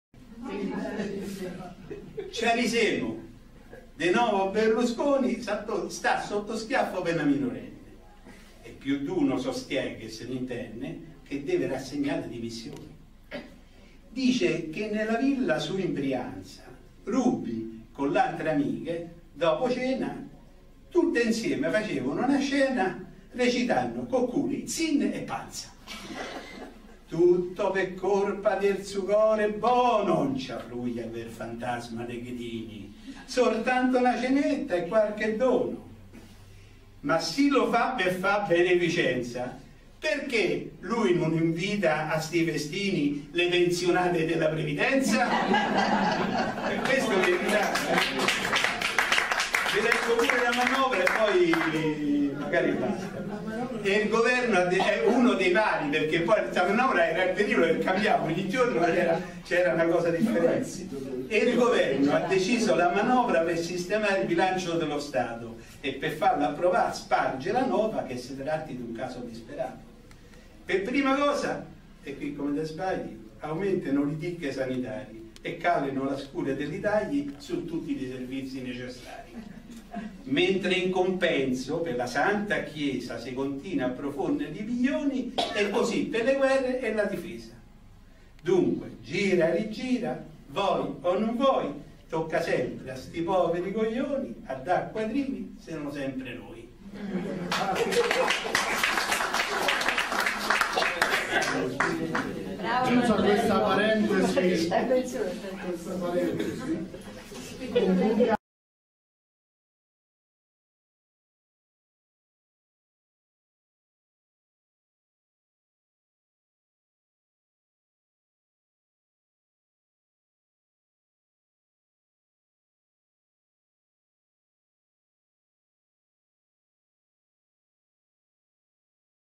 RUBBY più LA MANOVRA, sonetti di Marcello Grassi
Serata belliana presso il Circolo SEL Alberone di Roma, 23 novembre 2011, con alcuni sonetti romaneschi del Prof. Dott. Marcello Grassi.